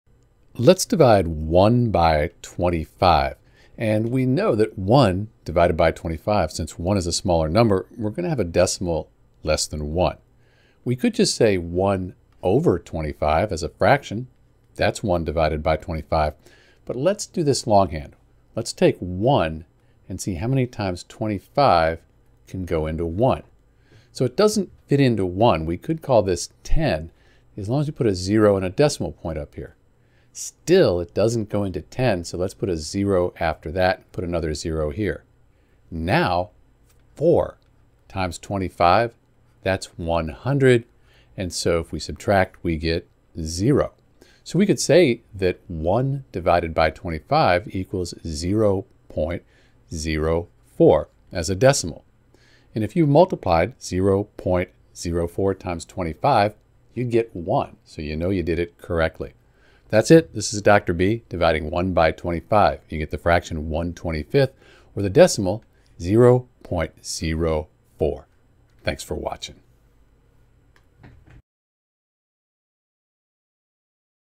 1 divided by 25 (1 ÷ 25)
To divide 1 by 25 (1 ÷ 25), we can follow the process for division:

The general process for division:

Make an educated guess.
Multiplication.
Subtraction.
Bringing down the next digit (when necessary).
For 1 divided by 25 (1 ÷ 25):

You can check your work by multiplying the quotient (0.04) by the divisor (25). If the result is 1, you've correctly performed the division.

Remember, when learning to divide numbers, it's a good practice to use a calculator to verify your work and learn from any mistakes made during the division process.